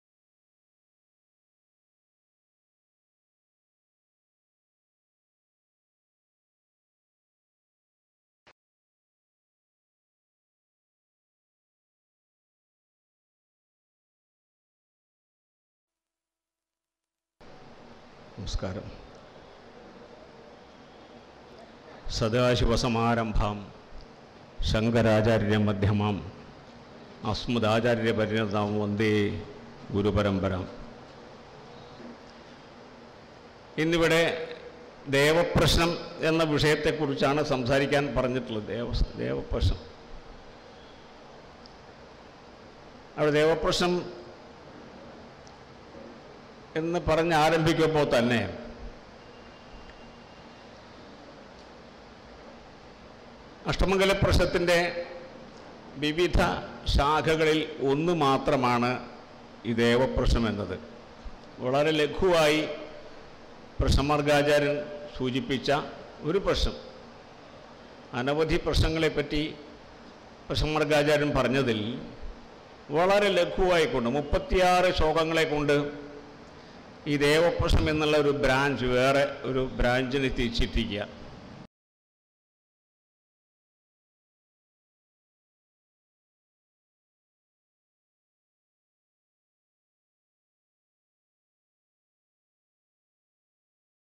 78th Astrology Seminar Class Video available | Deva Prasnam @ Shri Kootanad Ravunni Panicker
Astro-Vision's 78th astrology seminar class video was available for purchase. Here the video, look at promo speech of Shri Kootanad Ravunni Panicker about the topic Deva Prasnam.

Also to find the upcoming astrology master and seminar class details, call this phone number to pre-register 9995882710, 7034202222